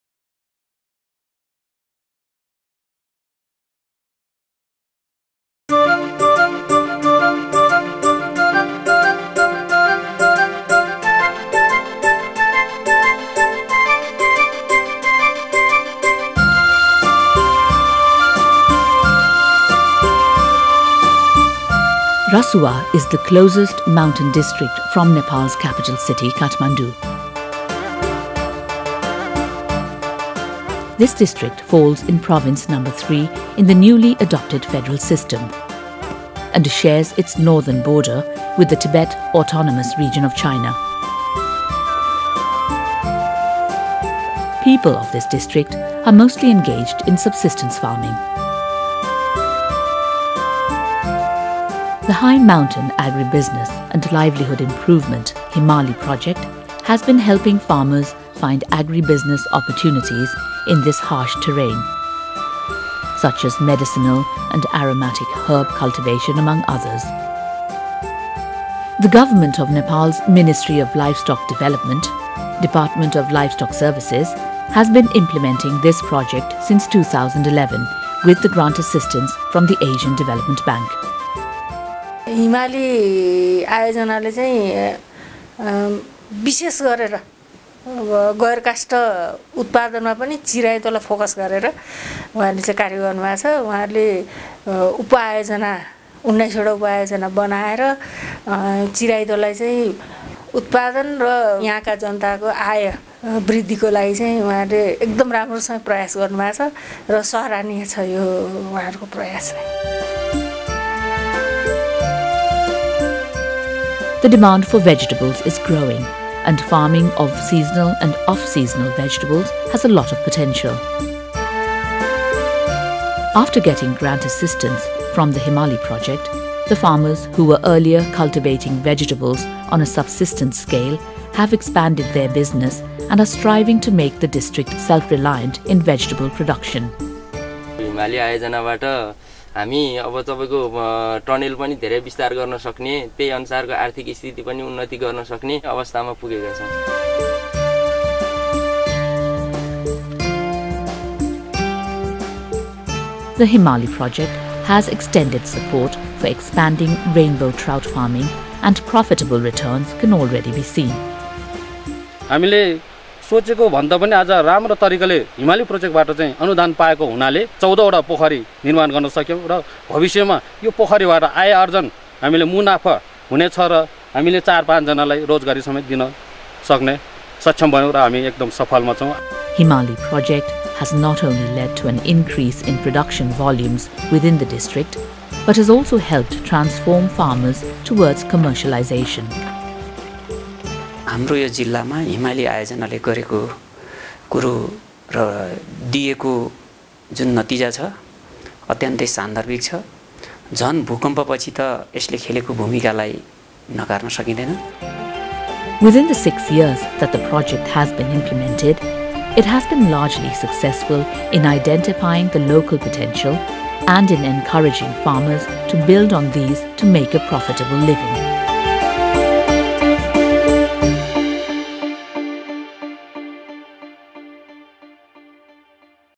HIMALI Project in Rasuwa district, Nepal (Short Version English)
The High Mountain Agribusiness and Livelihood Improvement Project (HIMALI) has developed groundbreaking ways to initiate and establish agribusiness in the high mountainous areas of Nepal. HIMALI is implementing though coordination of several stakeholders, including farmers, elders, women, community organizations, research organizations, public and private sectors from the district level up to central level. In Rasuwa district, Nepal, HIMALI project support the smallholder farmers to conduct own model of commercial farming, including agribusiness entrepreneurs to prepare quality business plans at district level. Inclusive sustainable development and climate change adaptation also contribute to good agribusiness practices.